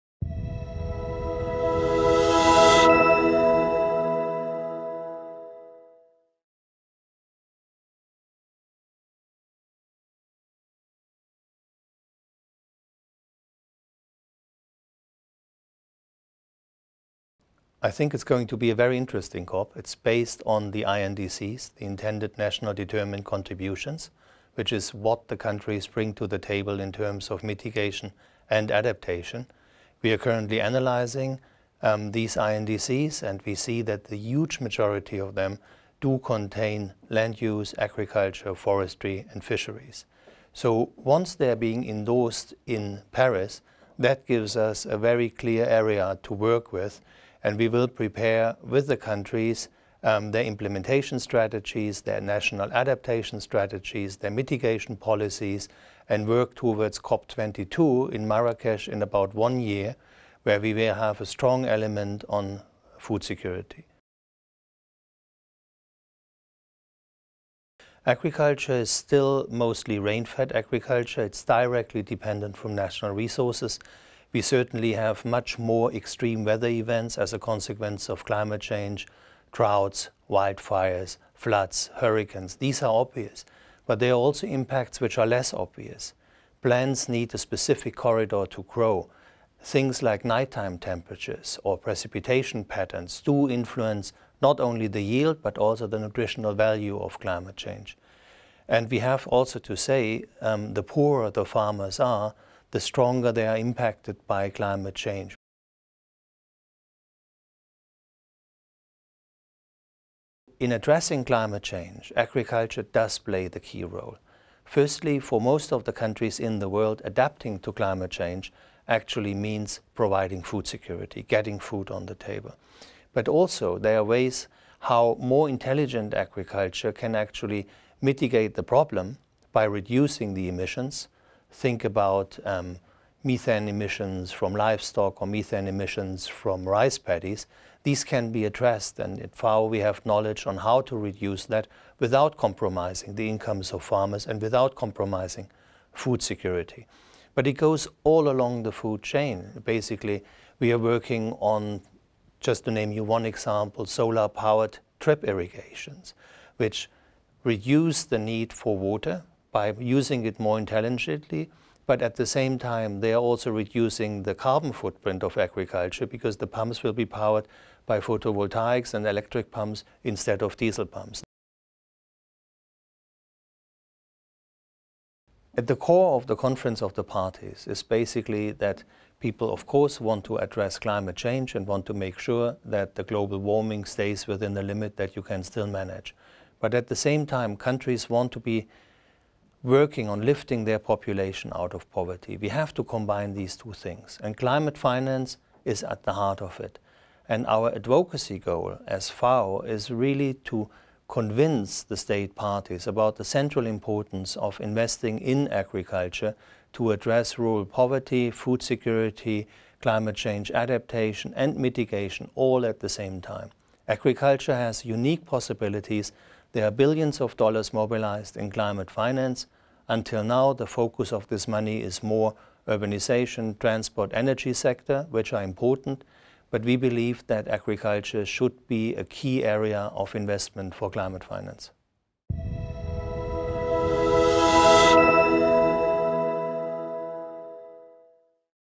Climate change: FAO at COP21
“In addressing climate change, agriculture does play the key role. For most countries in the world, adapting to climate change actually means providing food security.” Martin Frick talks about how the endorsement of the INDCs (Intended National Determined Contributions) at COP21 will give FAO the opportunity to work with countries, preparing the implementation, national adaptation strategies and the mitigation policies and look at COP22 next year in Marrakesh, where there will be a strong element on food security. He also underlines the importance of investing in agriculture in order to address rural poverty, food security, climate change adaptation and mitigation, all at the same time.